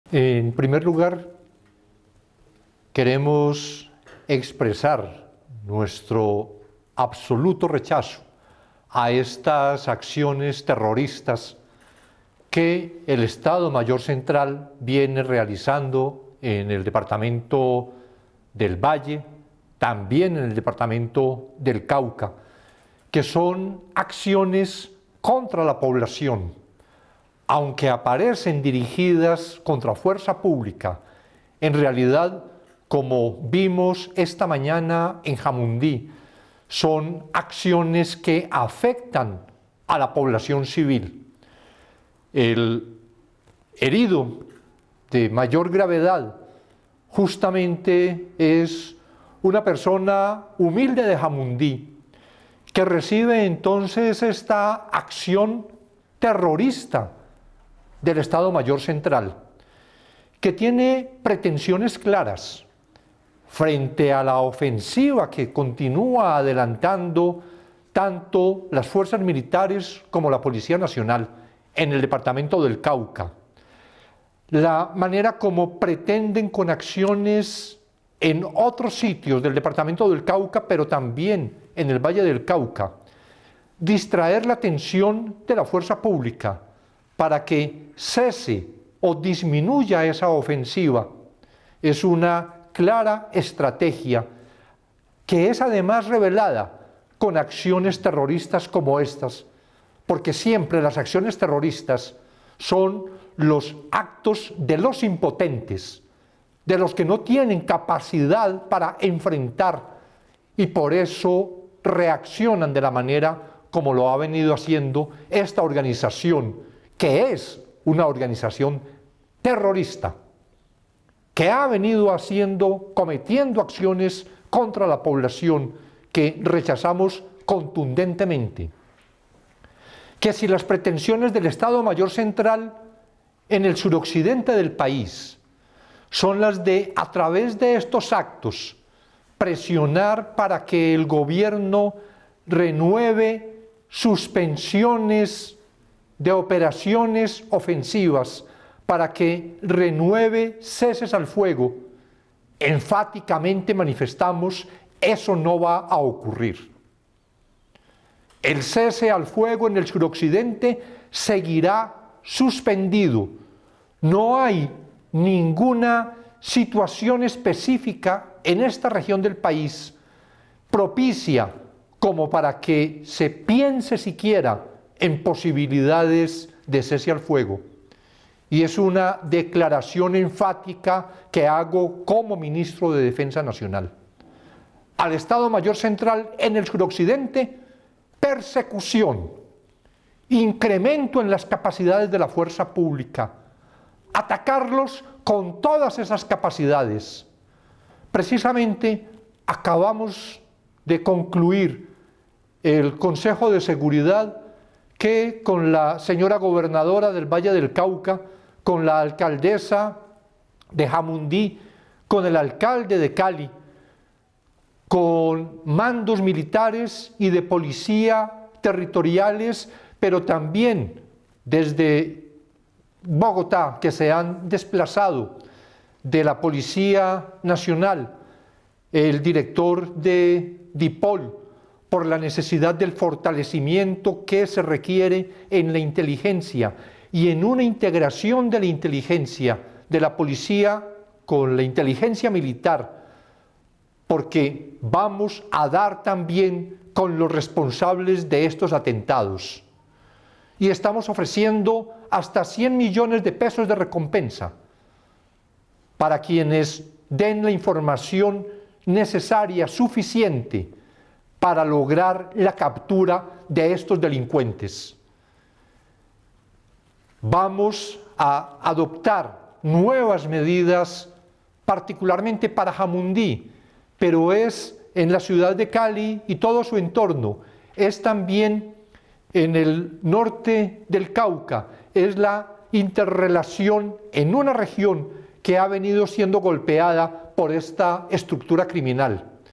Declaración Ministro sobre situación de orden público en Jamundí
Luego del consejo de seguridad, el ministro Iván Velásquez rechazó los atentados terroristas del ‘Estado Mayor Central’ en Cauca y Valle del Cauca, y enfatizó que estas acciones son en respuesta a la ofensiva que adelantan las Fuerzas Militares y la Policía

 Así mismo, indicó que se integrará la inteligencia de la Policía Nacional
 con la de las Fuerzas Militares en Cauca y Valle del Cauca.